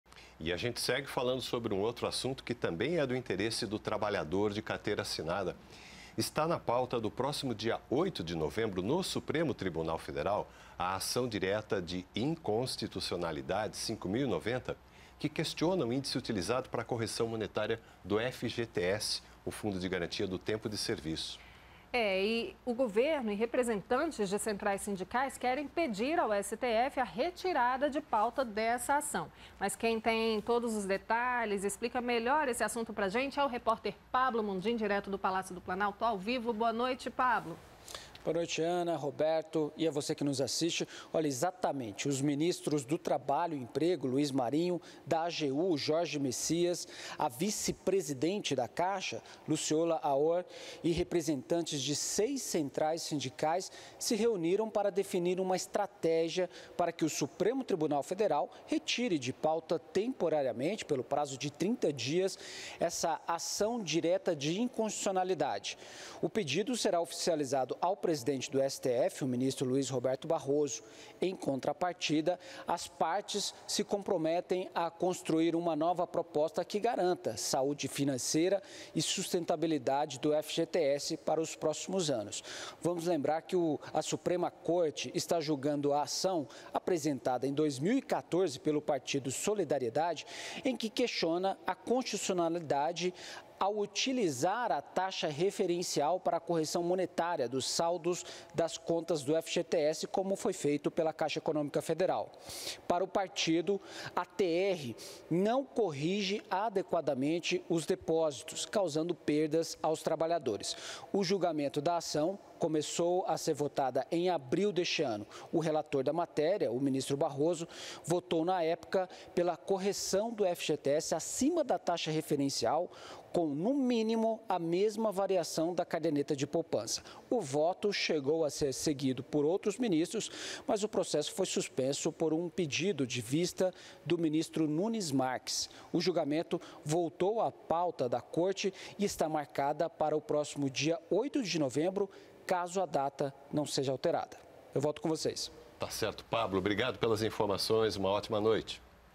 GOVERNO QUER QUE STF RETIRE DE PAUTA AÇÃO QUE QUESTIONA ÍNDICE DO FGTS
O STF deve julgar no próximo dia 8 a Ação Direta de Inconstitucionalidade que questiona o índice de correção do FGTS. Governo e centrais sindicais querem pedir ao Supremo a retirada de pauta dessa ação.